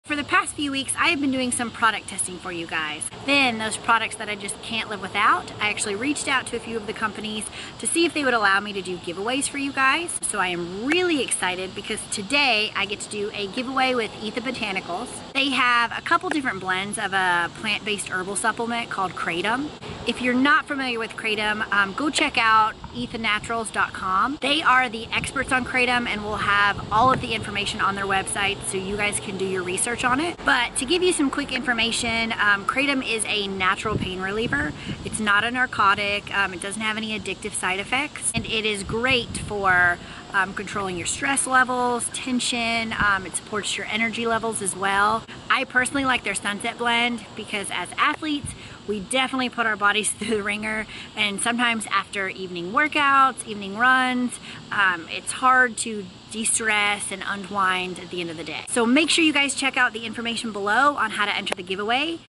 Bikini Competitor Emily Weiss Uses Best Kratom from ETHA
Fitness Entrepreneur and Bikini Competitor Emily Weiss uses ETHA SunSet kratom as part of her recovery process. Kratom also helps to relax after a long and stressful day.